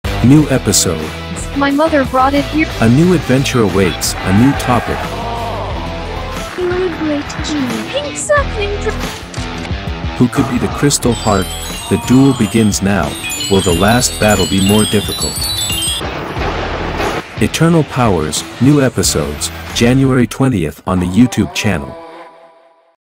Eternal Powers : New Episodes Trailer ( Fan-Made Marvel Series, 2023 )
Season episodes continue,
many exciting adventures await, fun, love, magic, who is crystal heart?!